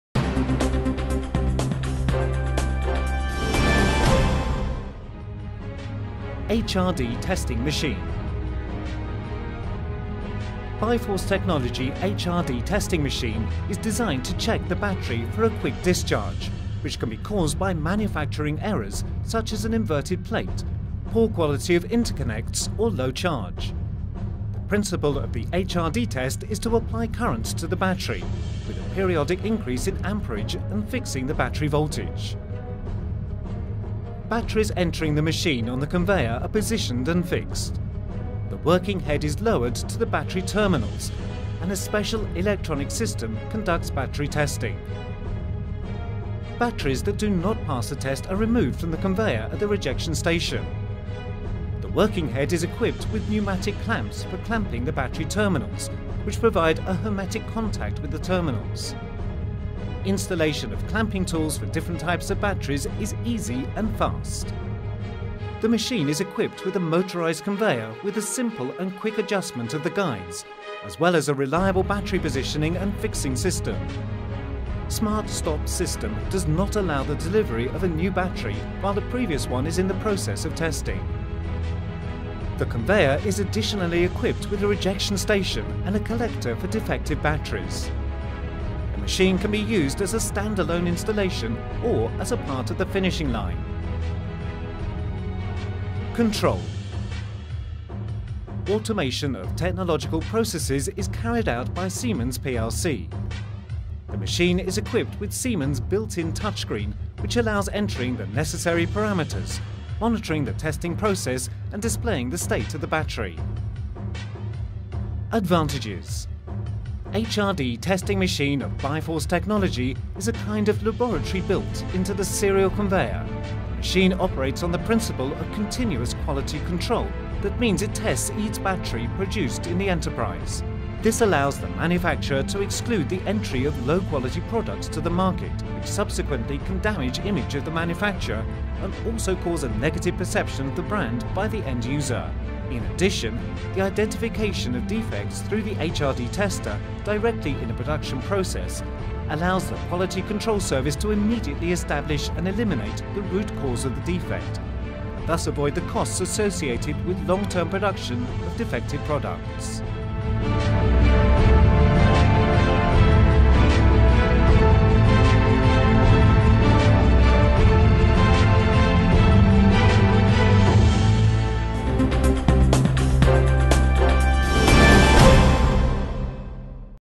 HRD Testing Machine | GT1.0
➡ BI-FORCE Technology HRD Testing Machine is designed to check the battery for a quick discharge. The principle of the HRD test is to apply current to the battery, with a periodic increase in amperage and fixing the battery voltage.

➡ Top 5 features:
1️⃣ Automatic process for testing batteries.
2️⃣ Rejection station for defective batteries.
3️⃣ Data collection with test results.
4️⃣ Compact design that saves space in the workshop.
5️⃣ No human intervention, reduced labor costs.